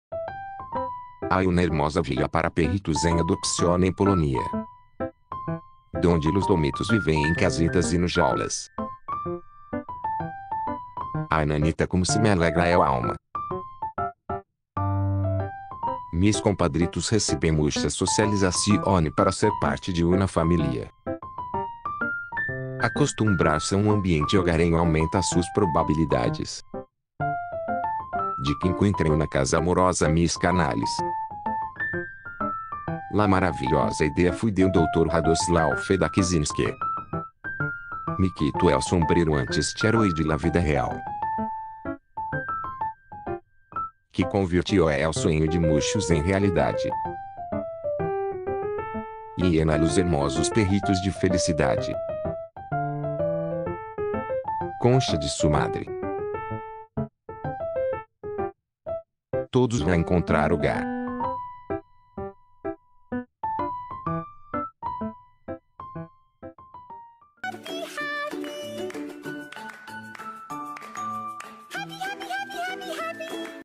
En este Refugio los Perritos Viven en sus Propias Casitas
En el Dog Village, los perritos adoptables viven en sus propias casas de campo en lugar de perreras. Reciben mucha socialización y formación para prepararlos para ser miembros de una familia.